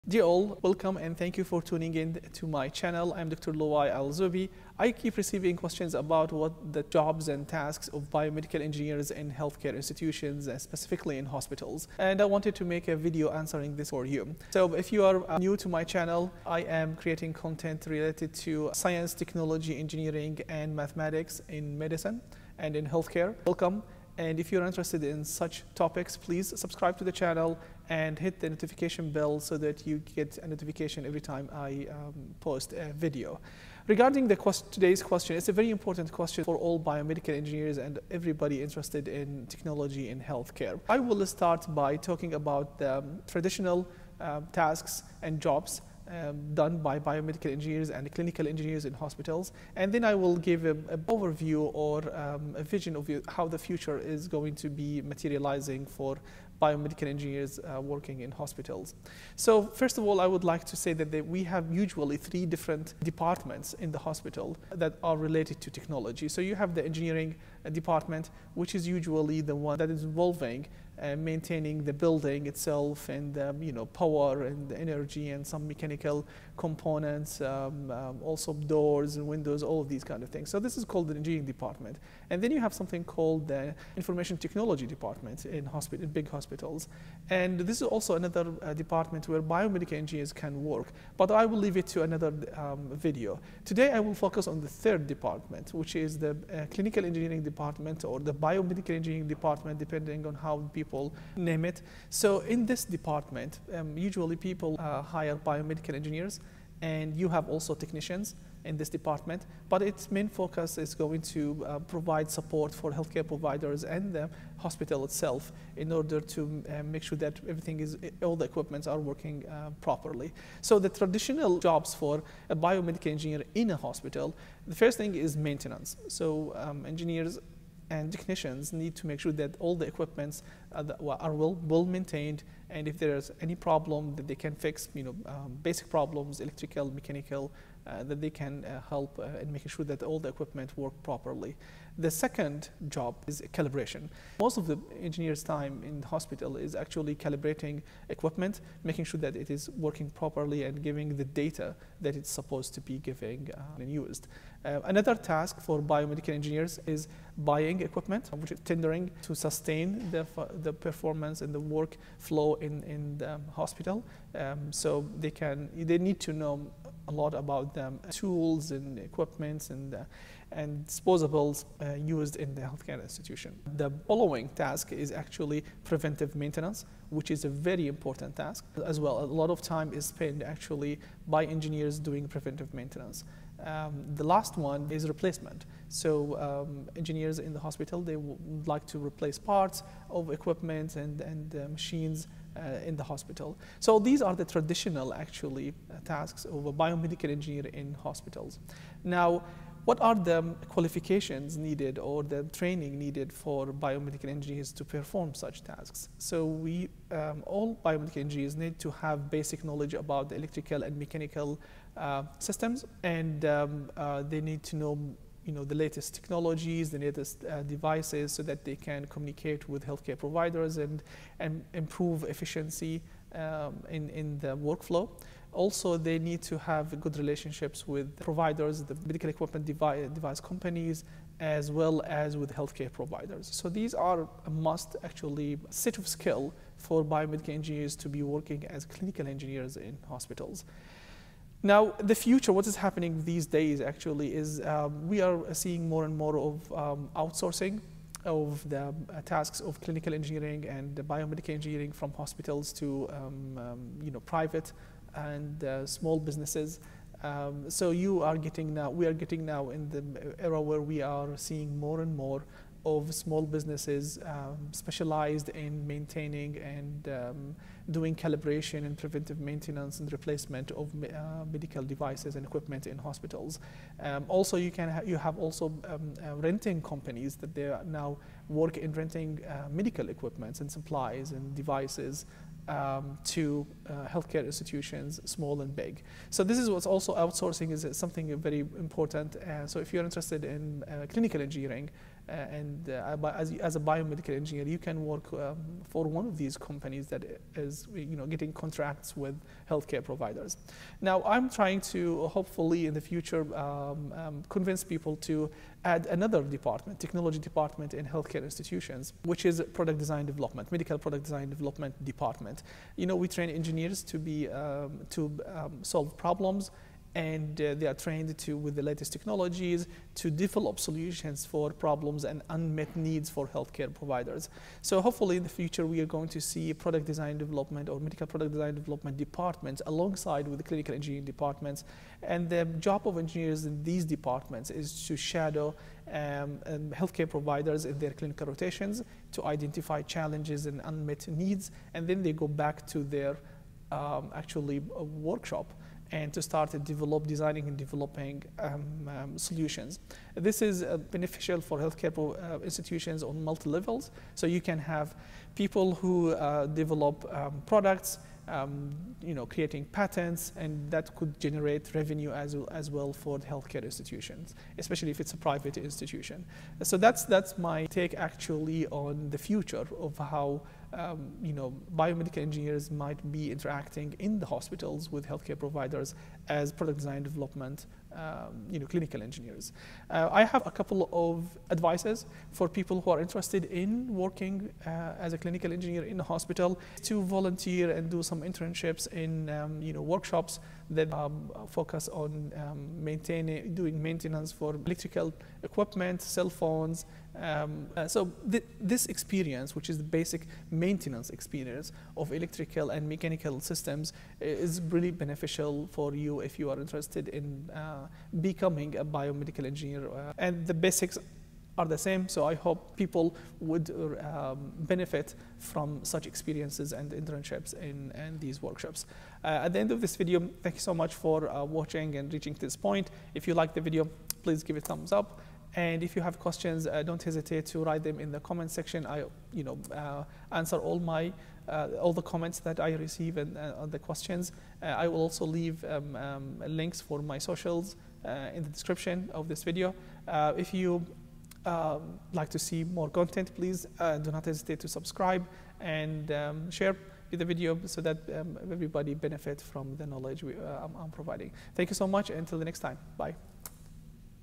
What Do Biomedical Engineers Do in Hospitals and in Healthcare Institutions? | A Futuristic View
Roles of Biomedical Engineers in Healthcare Institutions: Biomedical Engineering Careers, Explained! . This video presents and explains the jobs, tasks, and roles of biomedical engineers in hospitals and healthcare institutions. This video explains what clinical engineering is and what biomedical engineers do in hospitals.
Traditional jobs and tasks of biomedical engineers in hospitals (clinical engineering departments):
- Maintenance of medical devices and equipment
- Calibration of medical devices and equipment
- Preventive maintenance
- Purchasing medical devices, equipment, products, and solutions

Biomedical Engineering with Dr. Loay Al-Zube
Clinical Engineering with Dr. Loay Al-Zube

0:00 – Introduction & Purpose of the Video
Dr. Loay AlZube welcomes viewers and explains the goal: answering common questions about biomedical engineers' roles in hospitals.

1:36 – What Is the Clinical Engineering Department?
Overview of the clinical engineering department and its critical function within healthcare institutions.

2:05 – Traditional Tasks of Biomedical Engineers in Hospitals
Breakdown of key roles: maintenance, calibration, equipment purchasing, preventive maintenance, and replacements.

3:20 – Required Skills and Training
Essential qualifications and interpersonal skills needed for biomedical engineers in hospital settings.

4:03 – Future Trends & Emerging Roles
Discussion of outsourcing trends and the potential for medical product design & development departments within hospitals.

If you found these videos helpful and you really want to give something, it's very much appreciated.

Dr. Loay Al-Zube